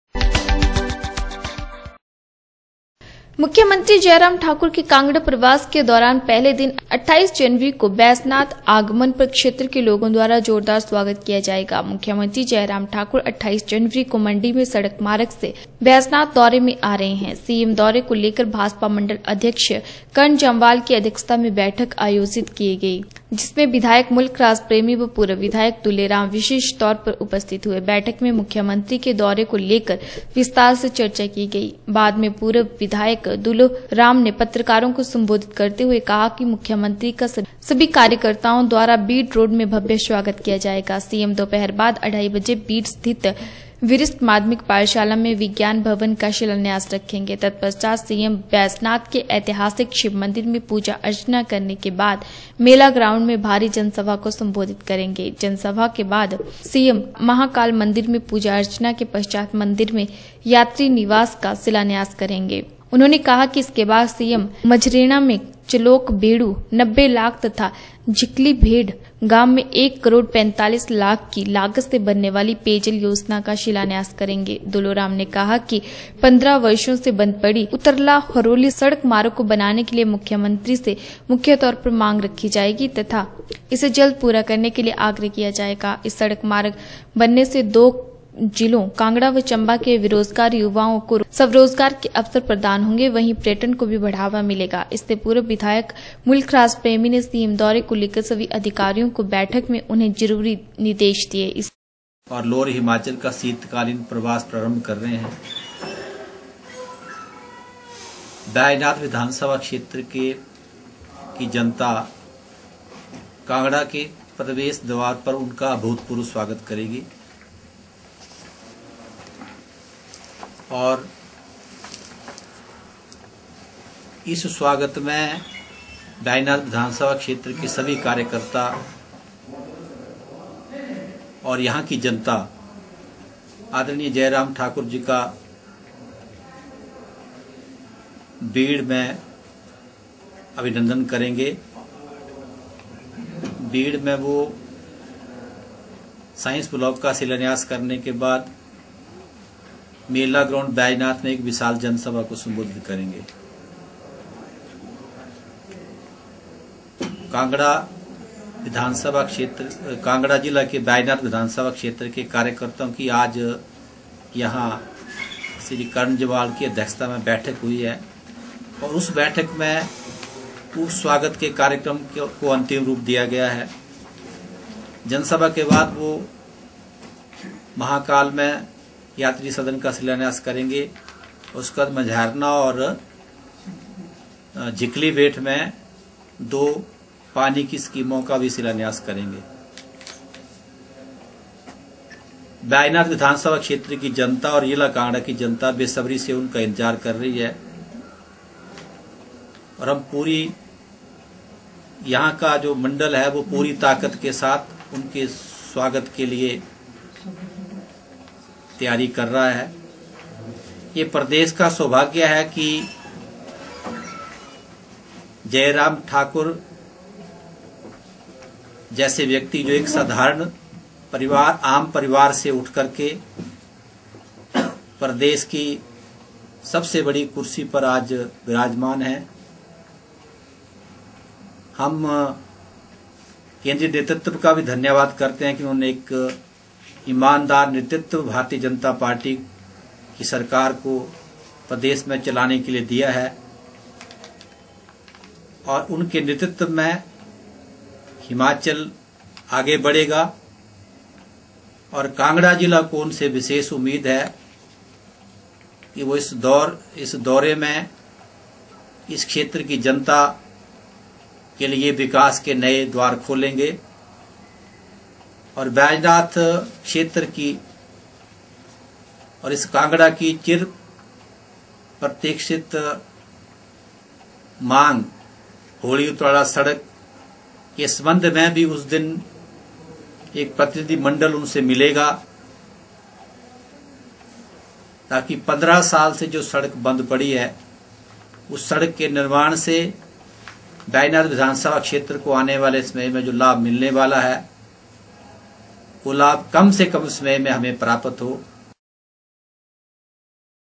मुख्यमंत्री जय राम ठाकुर के कांगड़ा प्रवास के दौरान क्षेत्र के लोगों द्वारा जोरदार स्वागत किया जाएगा।
मुख्यमंत्री जय राम ठाकुर के कांगड़ा प्रवास के दौरान पहले दिन 28 जनवरी बैजनाथ आगमन पर क्षेत्र के लोगों द्वारा जोरदार स्वागत किया जाएगा। मुख्यमंत्री जय राम ठाकुर 28 जनवरी को मंडी से सड़क मार्ग  से बैजनाथ दौरे में आ रहे है।सीएम दौरे को लेकर भाजपा मंडल अध्यक्ष कर्ण जम्वाल की अध्यक्षता में बैठक का आयोजन किया गया जिसमें विधायक मुलख राज प्रेमी व पूर्व विधायक दूलो राम  विशेष तौर पर उपस्थित हुए। बैठक में मुख्यमंत्री के दौरे को लेकर विस्तार से चर्चा की